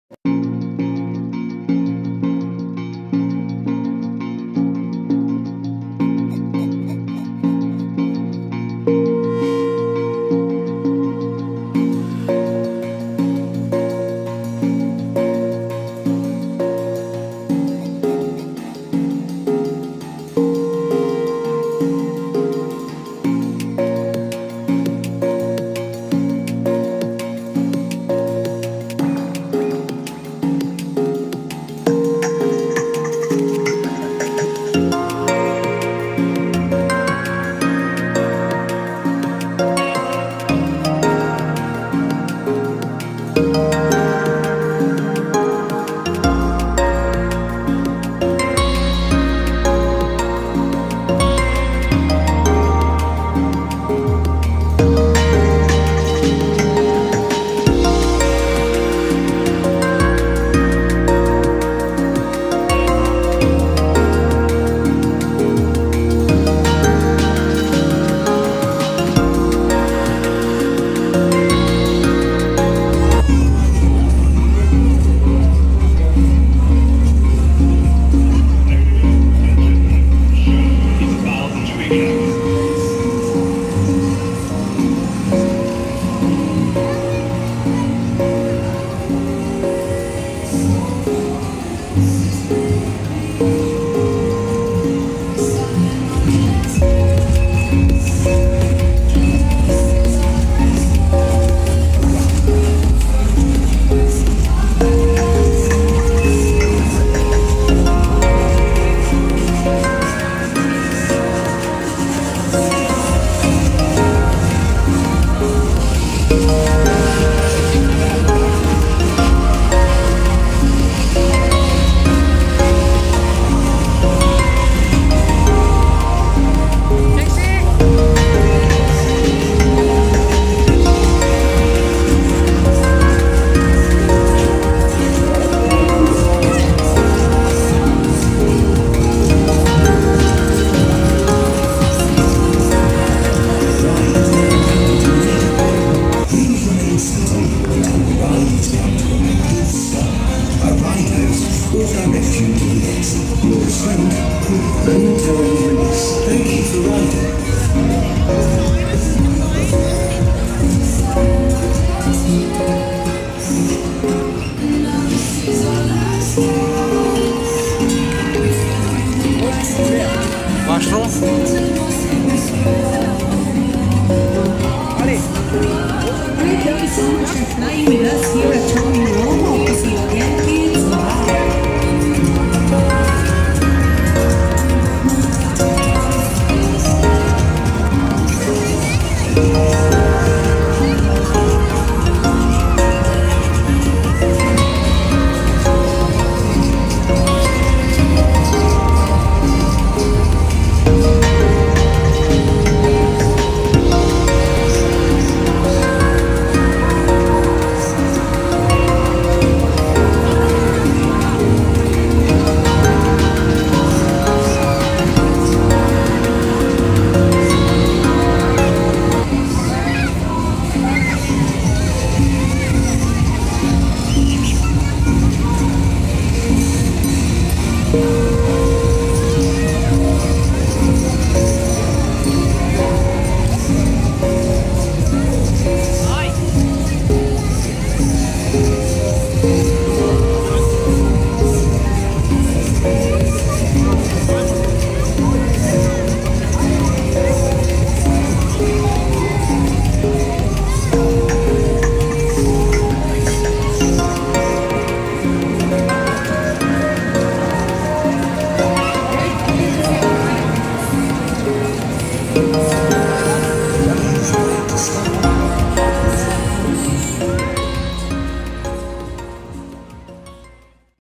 Global village Dubai 2019 | Best fun rides for kids
Global village is a seasonal attraction and shopping extravaganza held in the cooler winter. The carnival offers fairground rides for both adults and children, plus arcade games and games of skill